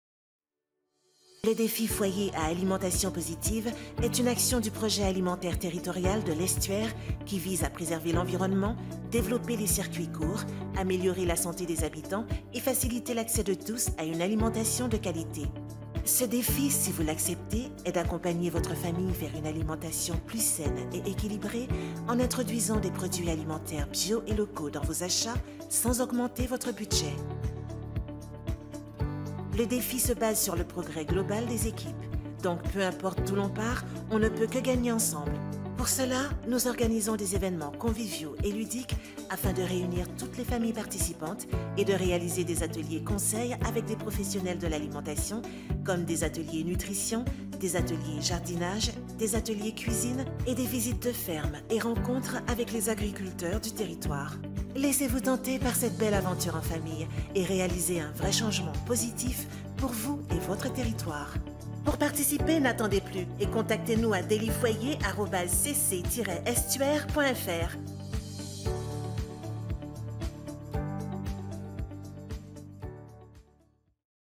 Le Projet Alimentaire Territorial - by CCE
Voici notre dernier projet réalisé. Il s'agit de mettre en avant le mieux manger et consommer sur les territoires, ici celui de l'Estuaire.
Ravis de collaborer sur ce projet plein de belles valeurs.